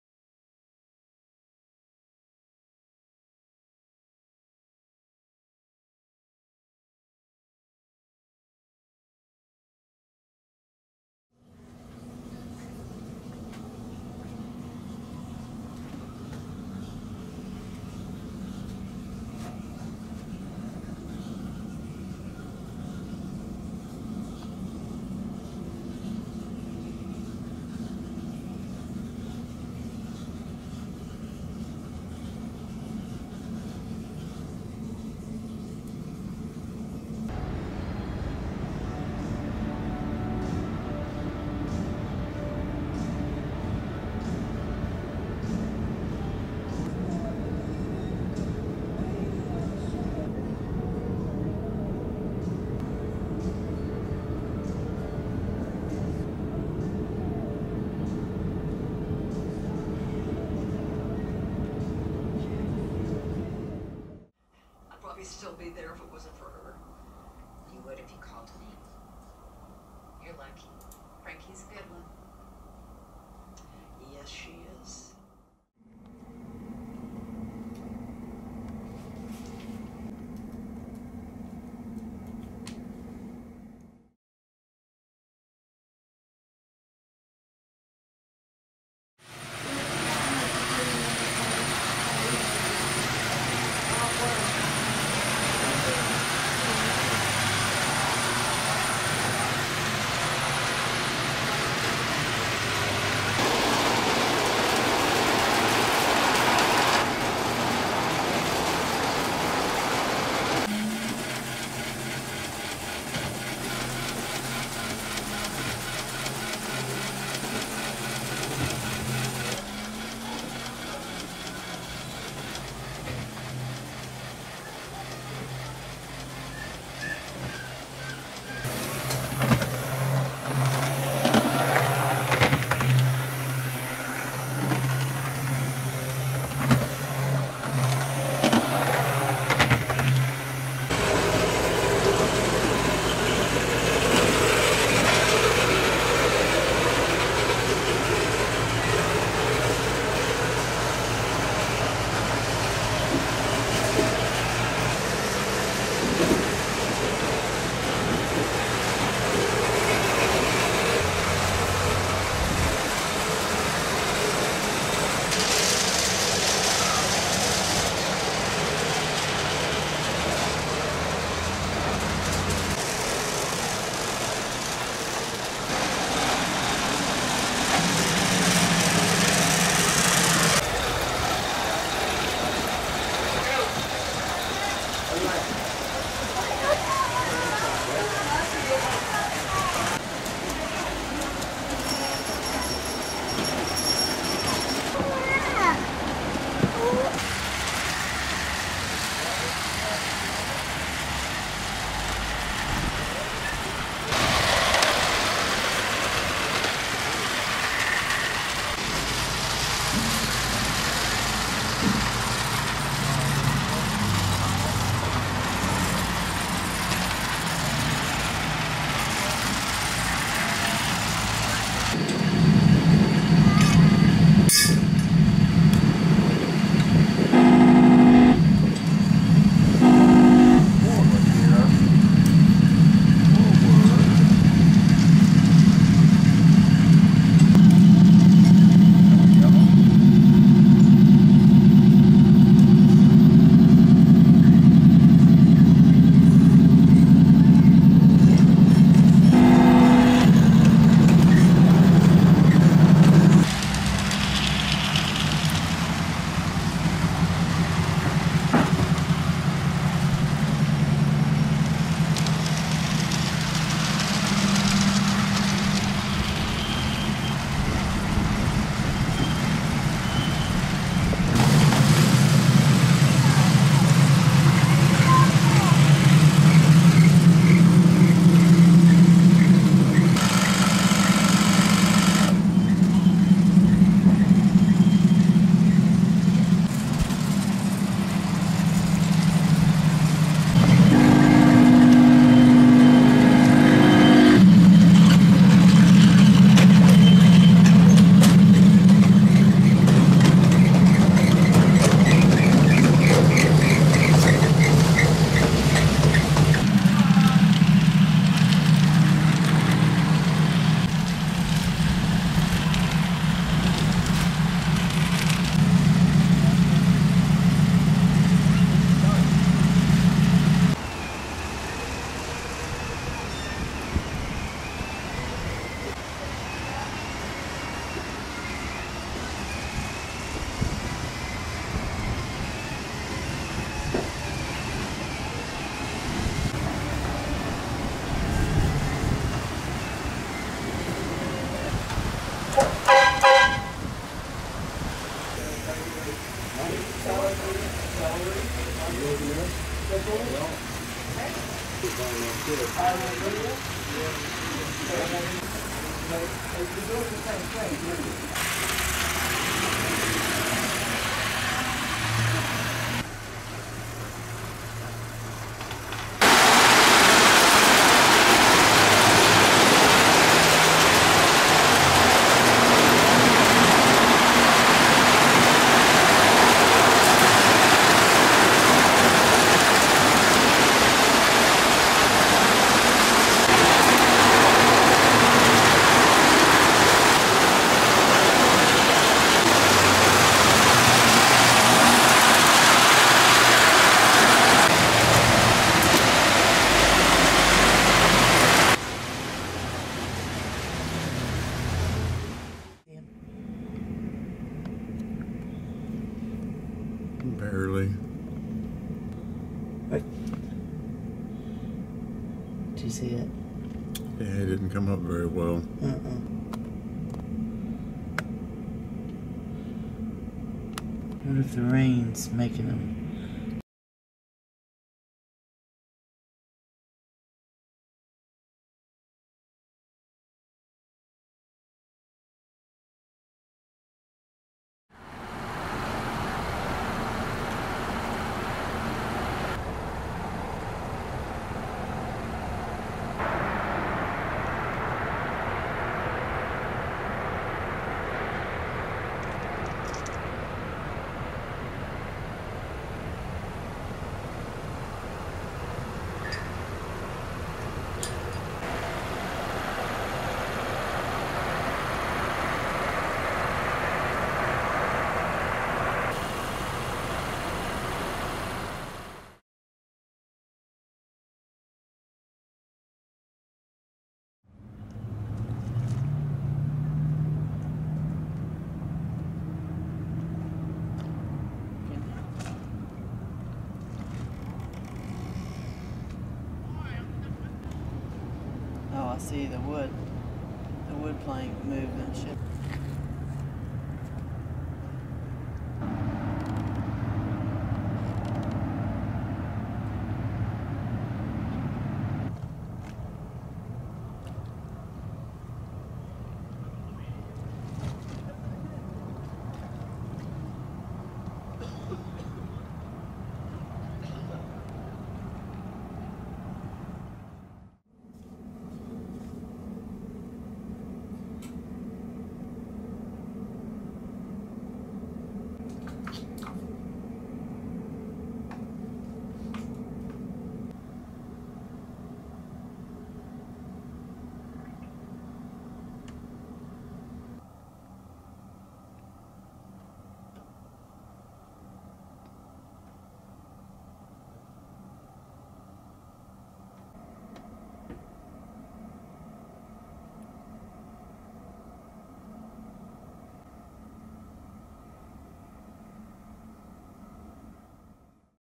Trip to Biloxi, Mississippi. January 15, 2020.
Biloxi Mississippi.  Stayed at the Beau Rivage.